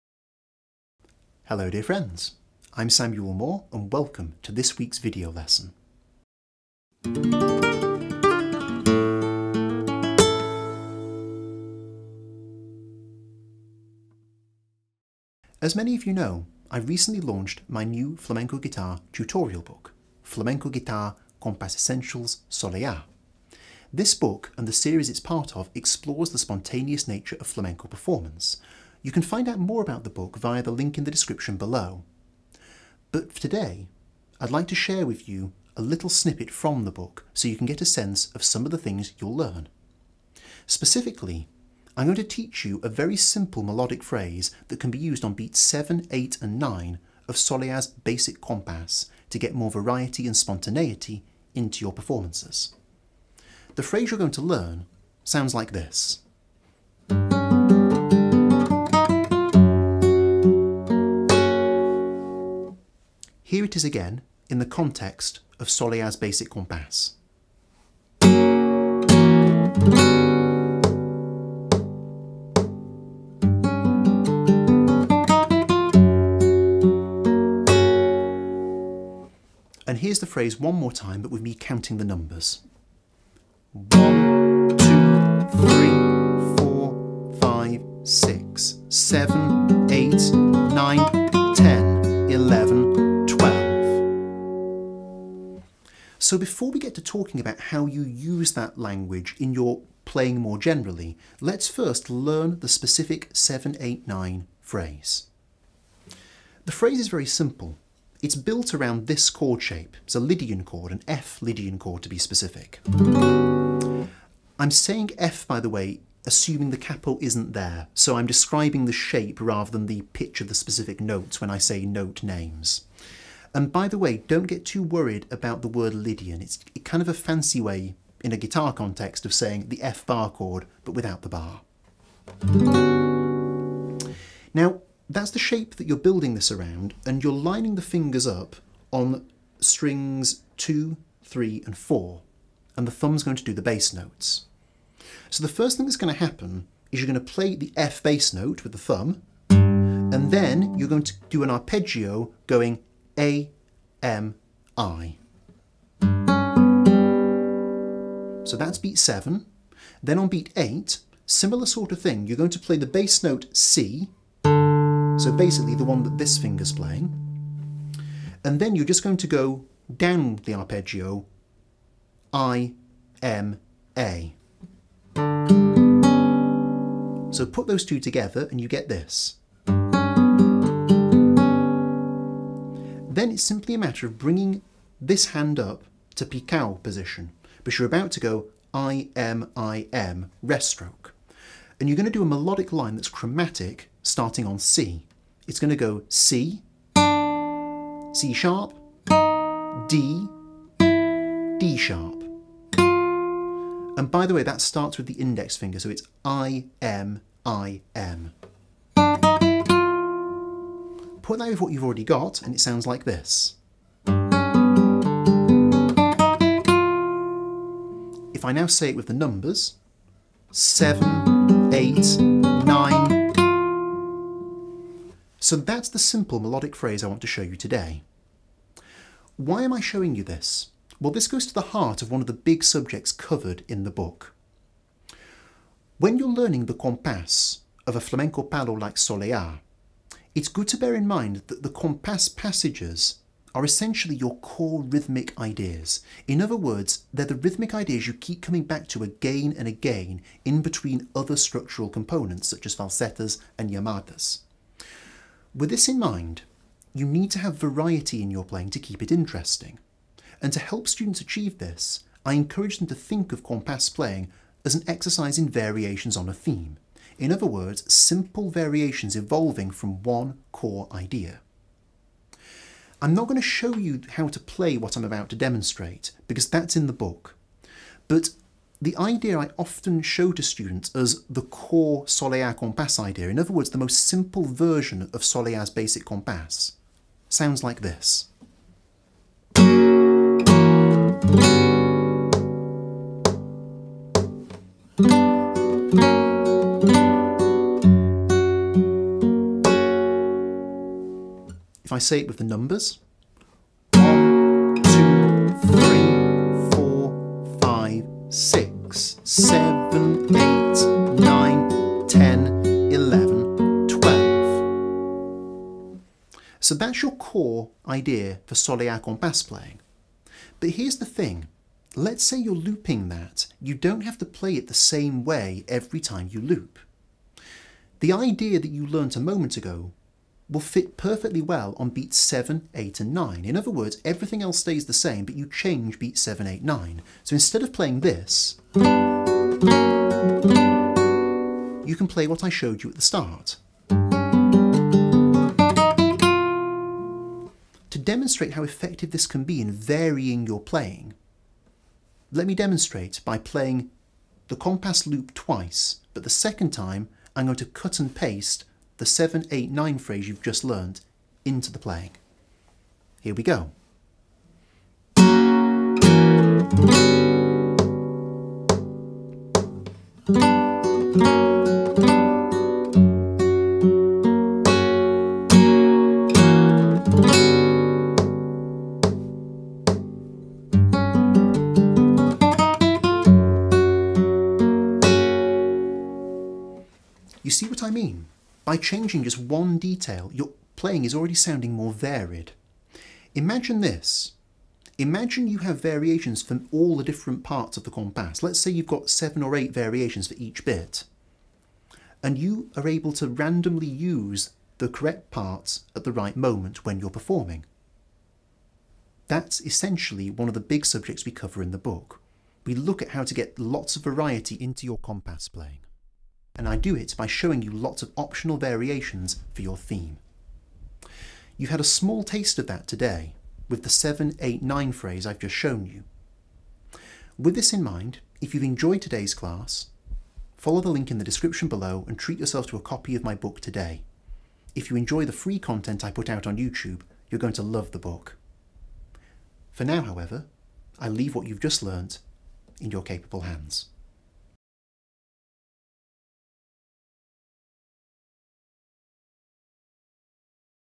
Essential Flamenco Language (Video 1) - A Useful Soleá Phrase
As part of the launch of my new flamenco guitar tutorial book (Flamenco Guitar, Compás Essentials: Soleá), I'm sharing a free lesson with you all today!